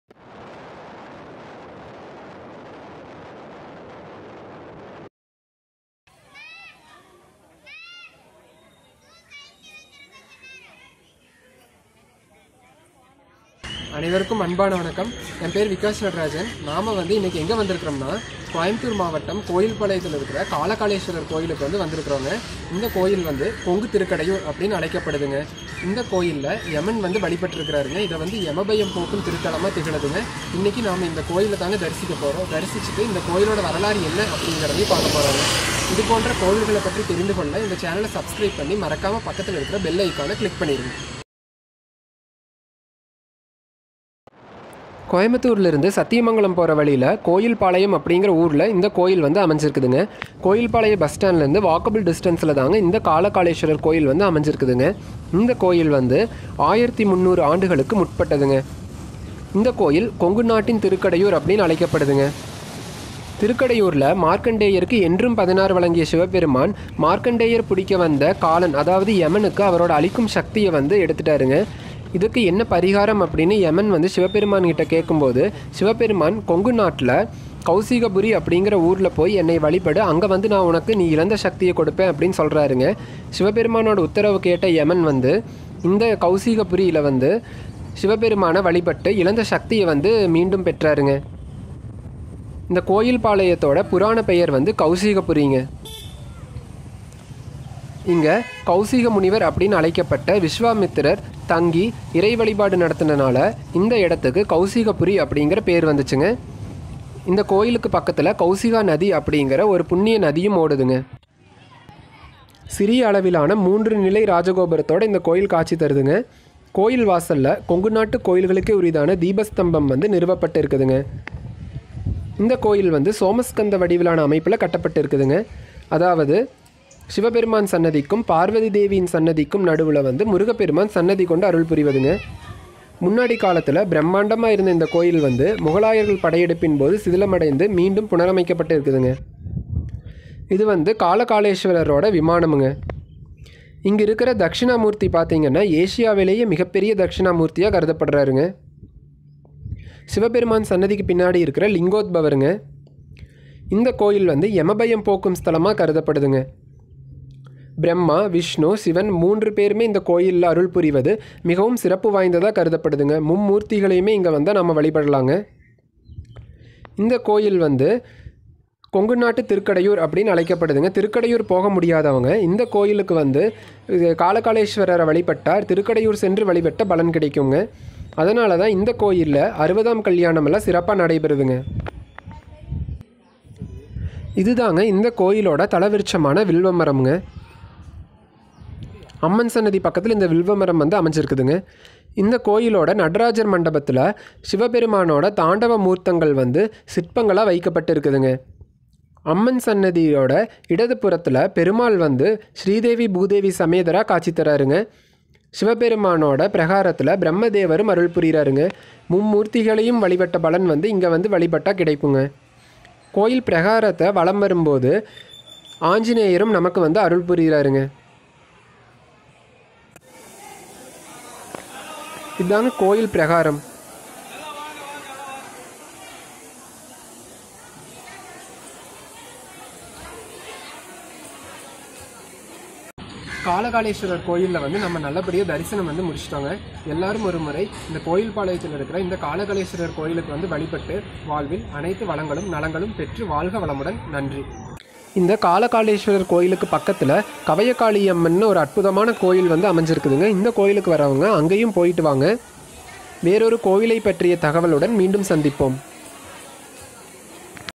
கொங்கு நாட்டின் திருக்கடையூர் | காலகாலேஸ்வரர் திருக்கோயில் | Kalakaleswarar Temple | Kovilpalayam
கொங்கு நாட்டின் திருக்கடையூர் என்று போற்றப்படும் அருள்மிகு கருணாகரவல்லி உடனுறை அருள்மிகு காலகாலேஸ்வரர் திருக்கோயில், கோயில்பாளையம், கோவை மாவட்டம்.

எமபயம் போக்கும் திருத்தலம்